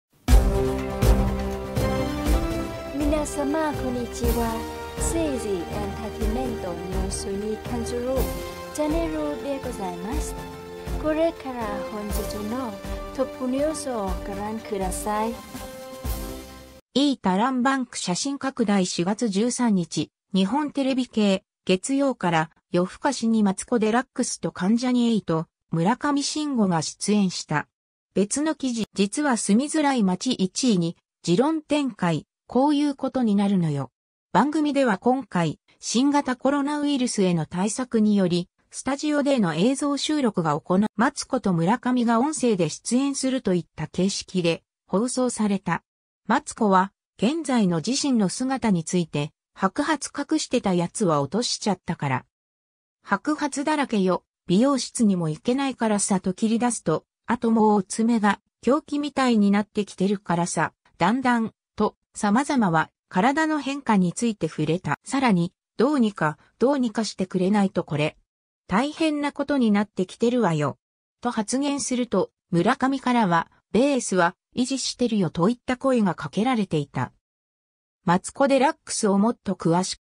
【緊急ニュース】 - 2020年04月16日 マツコ・デラックスが美容室通えず「白髪だらけよ」
芸能総合,マツコ・デラックス,新型コロナウイルス,新型肺炎,ニュース,